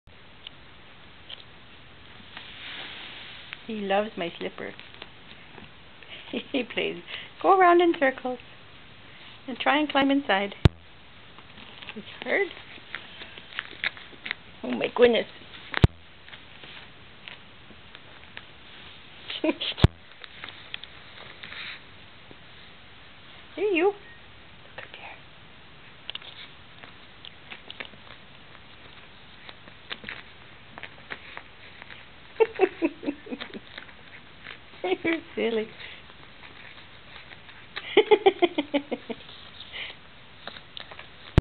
Buster likes my slippers
Silly Buster playing with slippers